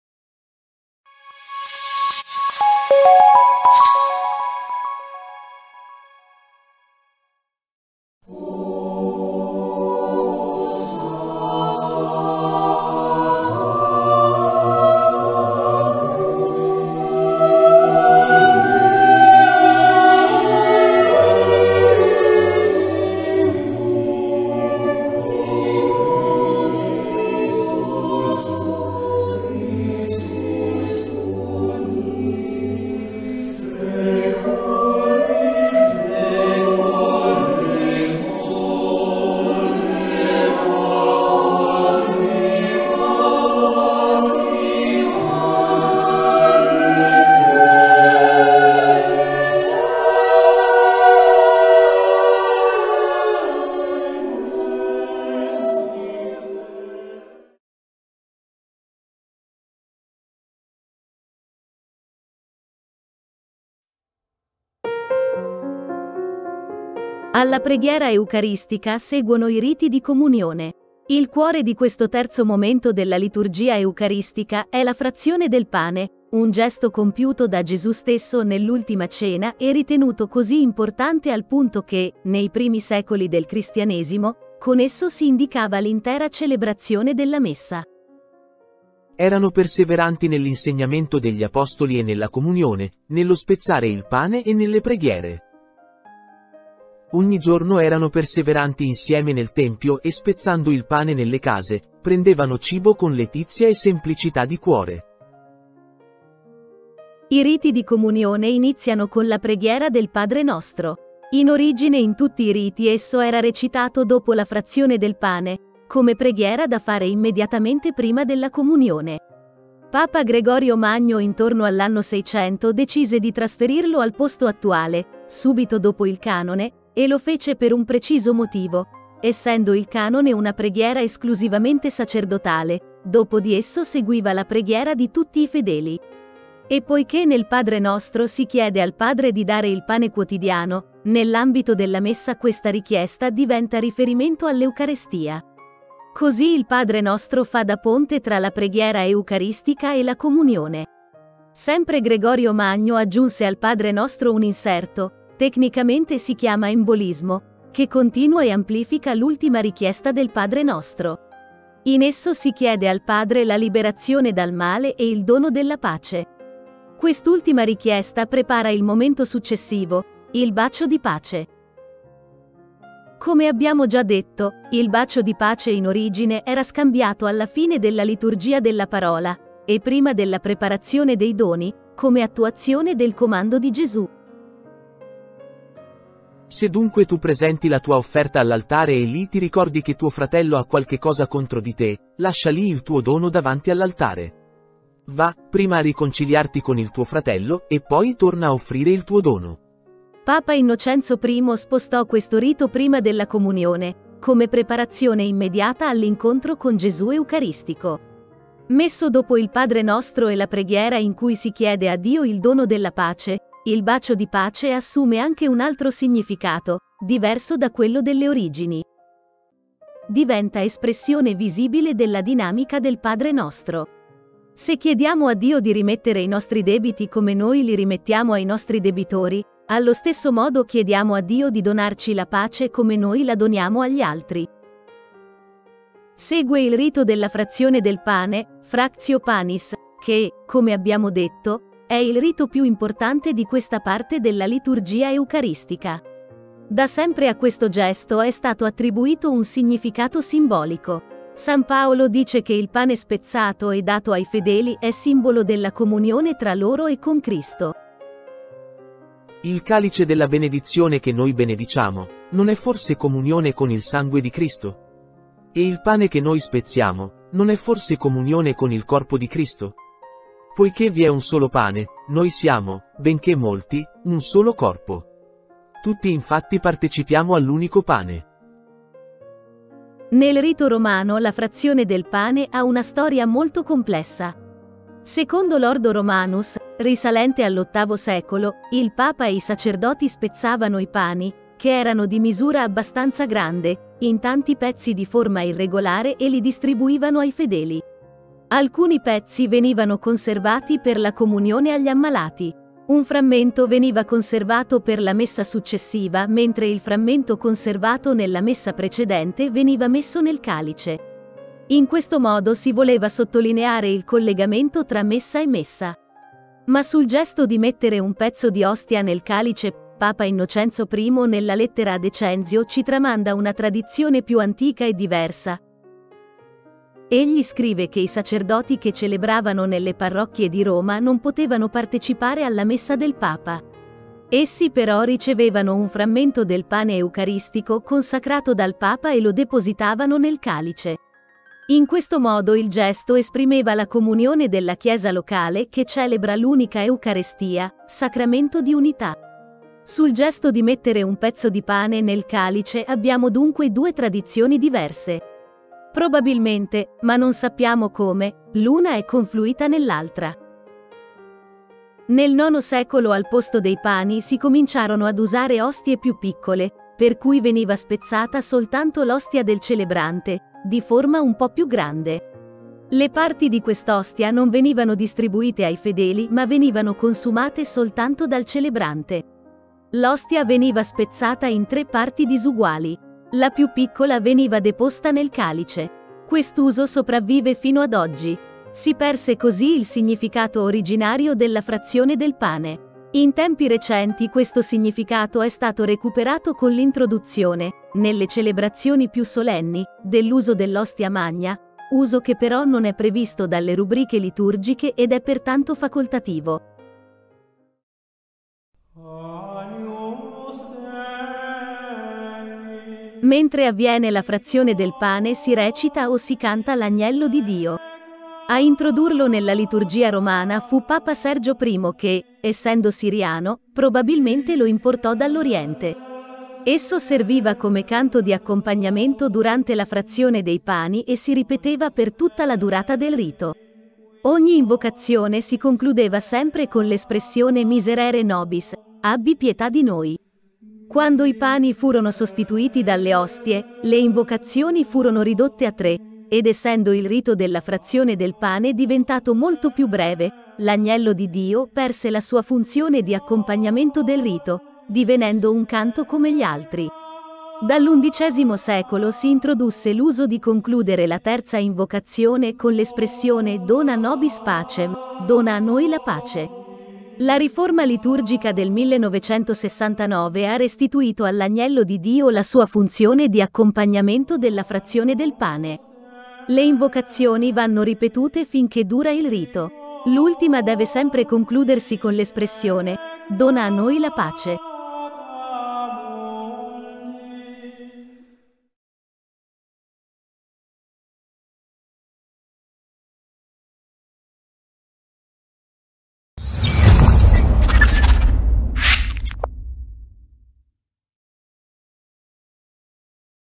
SPIEGAZIONE DELLA MESSA: evoluzione storica/4
In questo video esaminiamo il terzo momento della Liturgia Eucaristica: i Riti di Comunione.